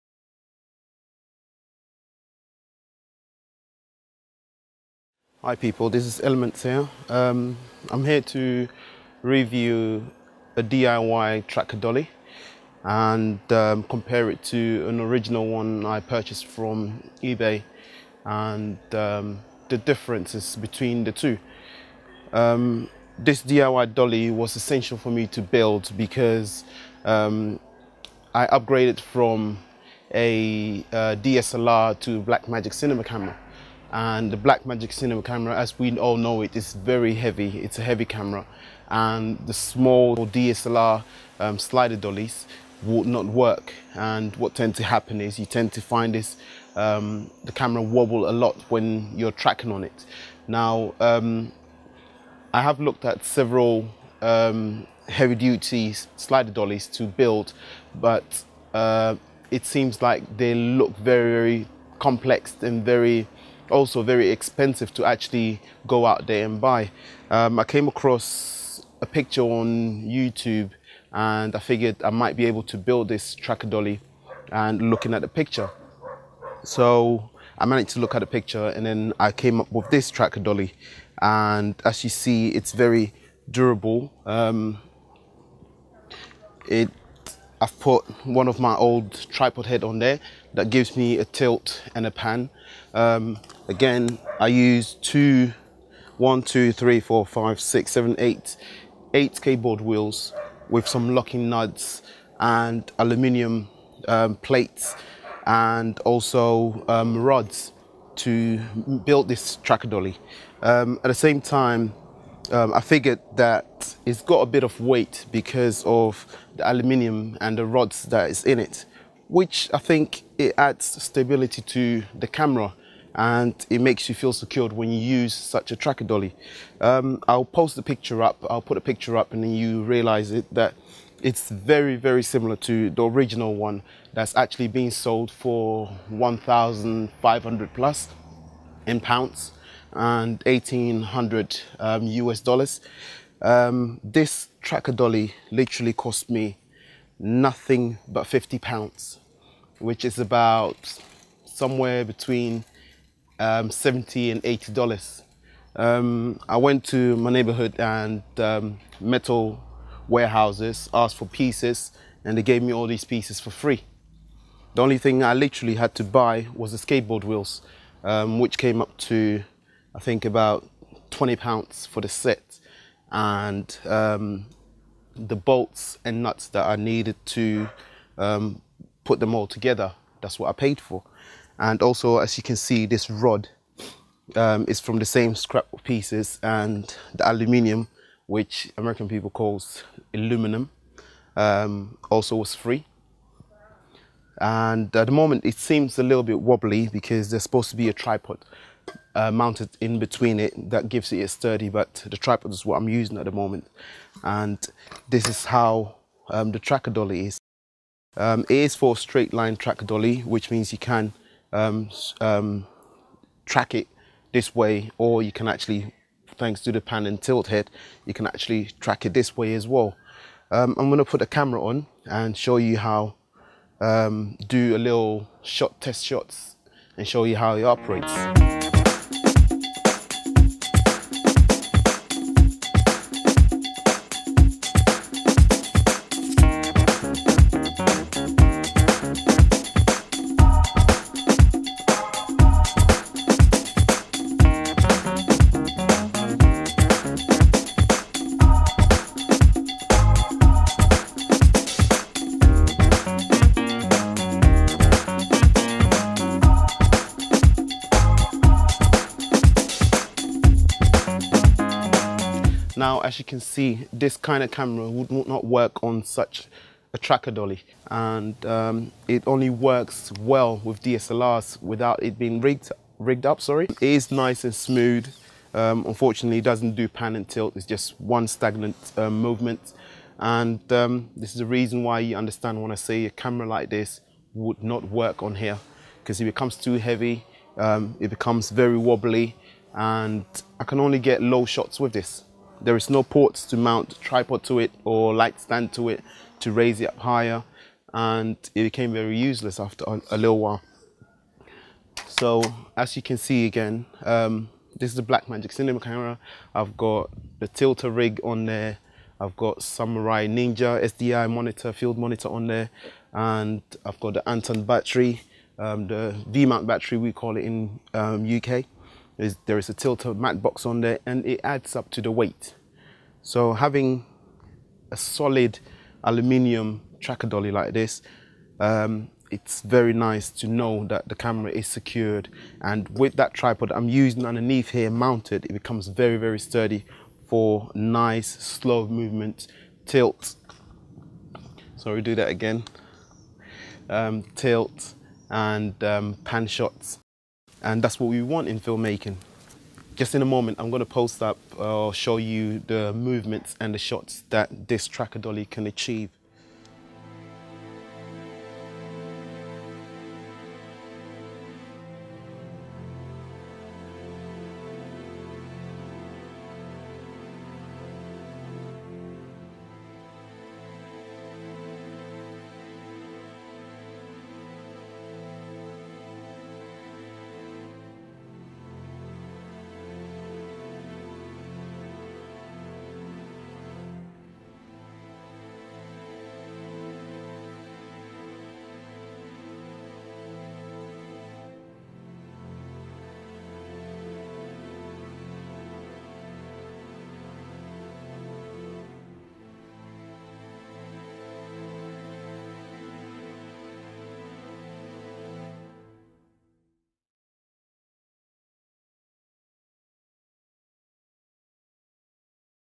DIY camera dolly - Film makers tools
Reviewing a Heavy Duty track dolly I built myself with scrap aluminium pieces. £50